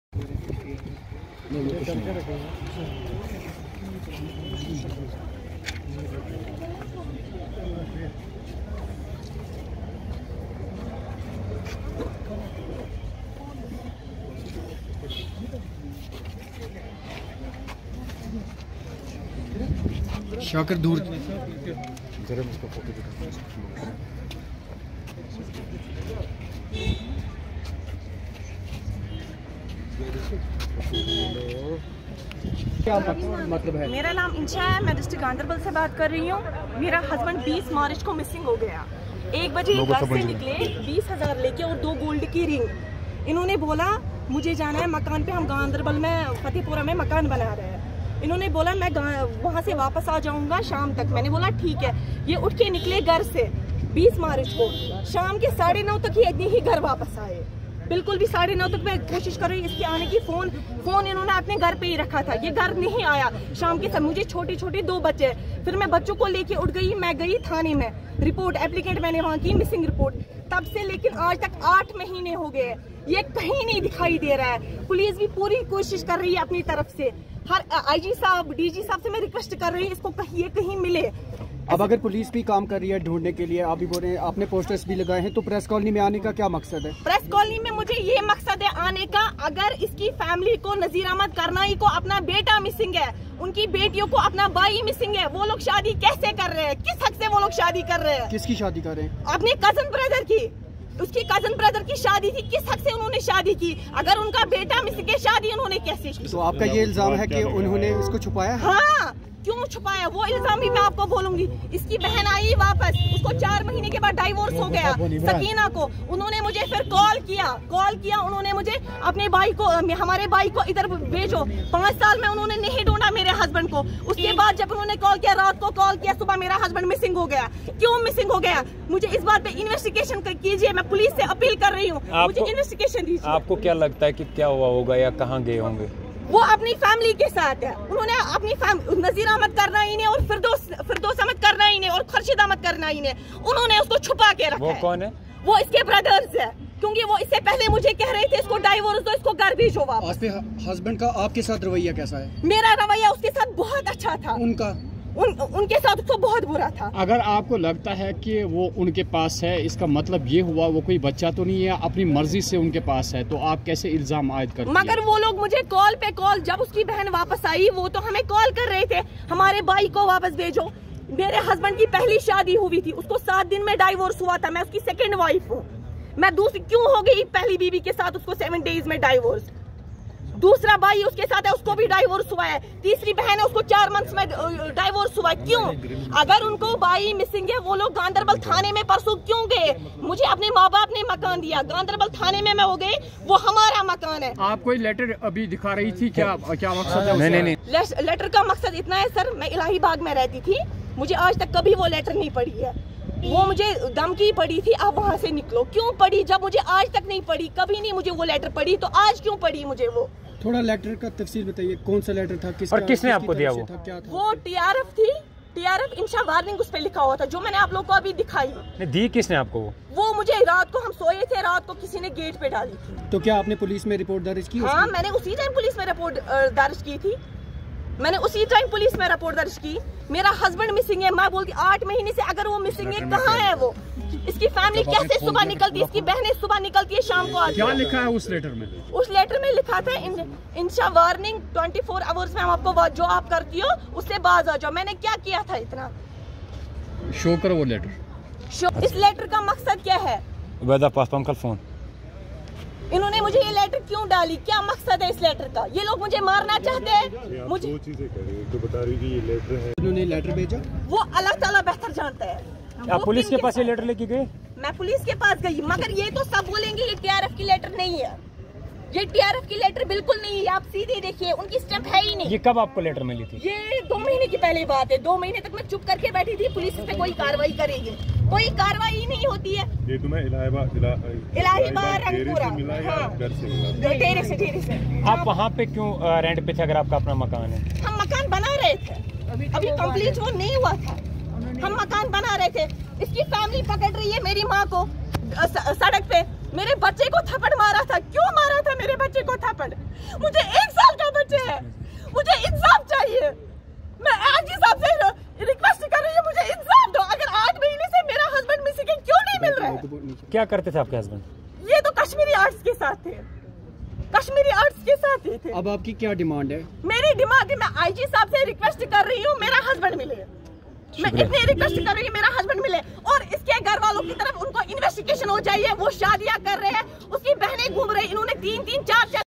Missing Man from Rangpora Illahibagh Soura Sgr, Reyaz Ahmad's Wife has appealed him to return back
Missing Man from Rangpora Illahibagh Soura Sgr, Reyaz Ahmad's Wife has appealed him to return home considering his little children and the entire family. His wife Insha Afzal, daughter of Mohd Afzal

Malla said that her husband left home on

22nd March 2022 along with 20,000 rupees &

a pair of golden rings. Since then reyaz hasn't returned home thus worrying his entire family.